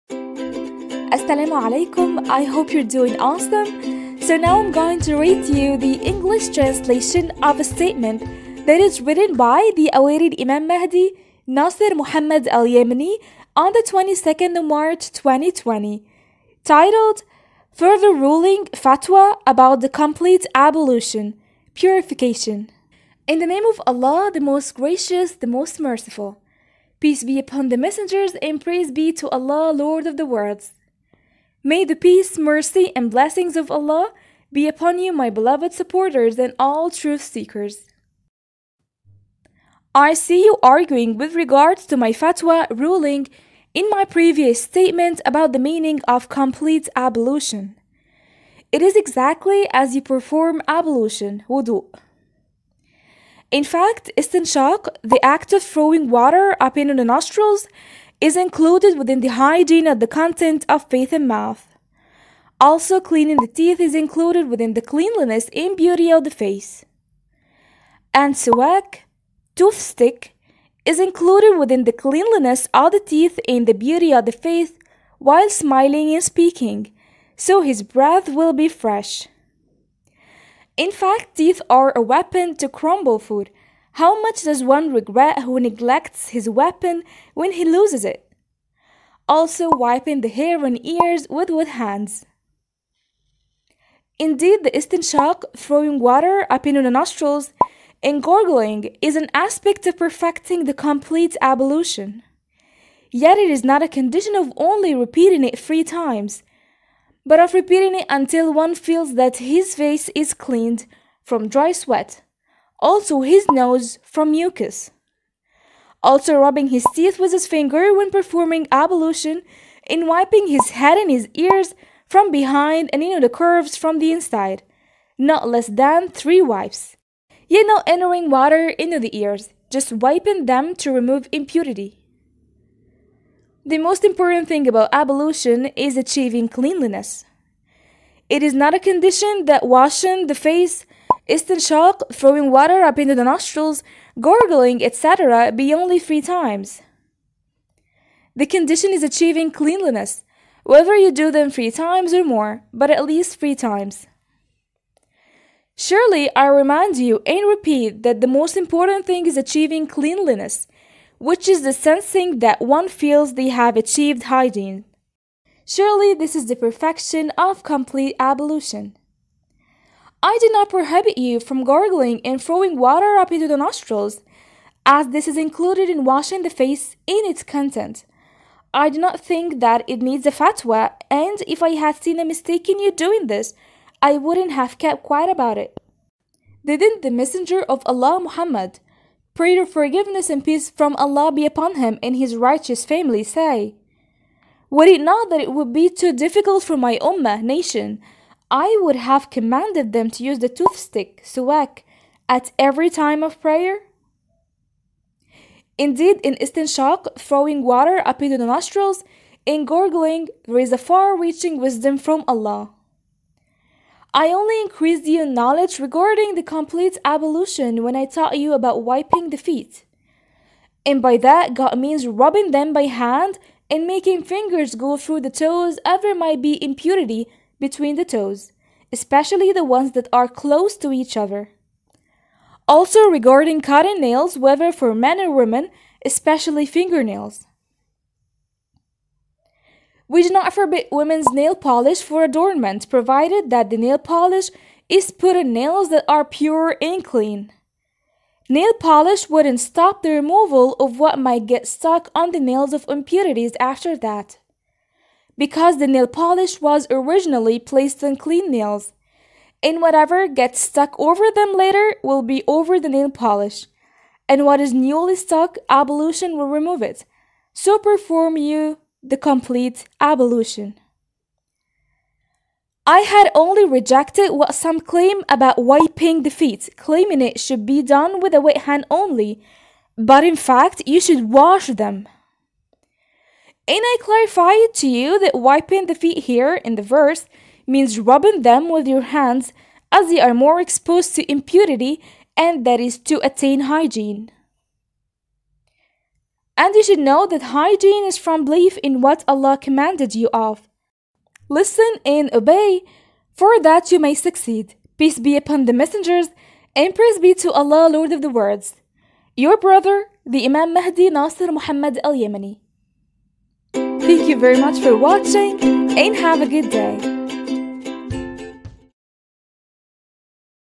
THE COMPLETE ABLUTION
What is the form of complete ablution?🤷‍♂️
And does nail polish really affect the validity of the act of ablution?💅
Also, is using the sewak (tooth pick) part of the process of ablution?🦷🤔